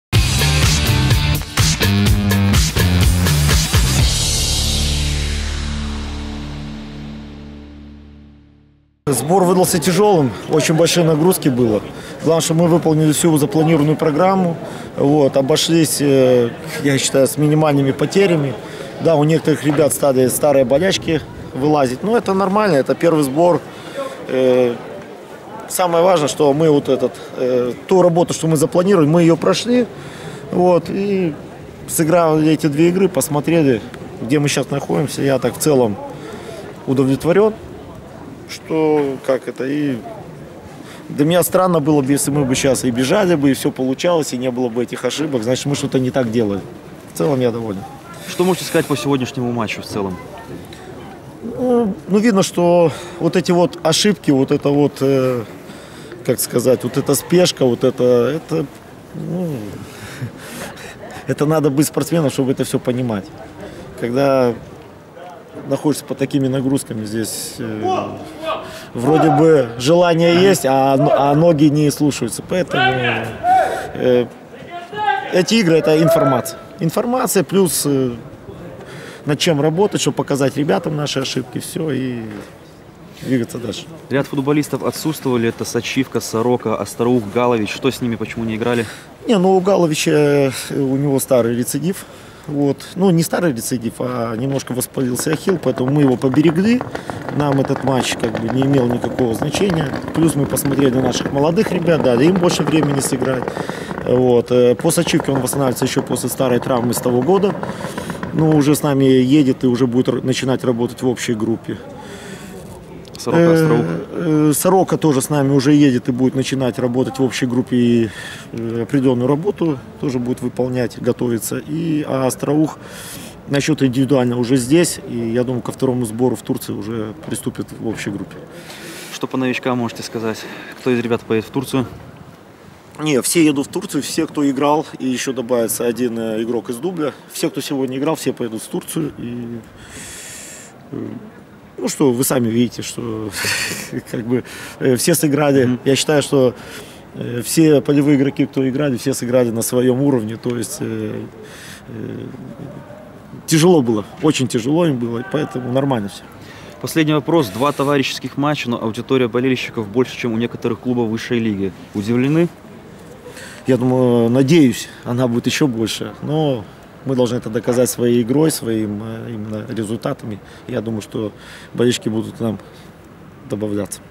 Сергей Гуренко: "Выполнили всю запланированную программу"
Сергей Гуренко подводит итог первому межсезонному сбору 2018.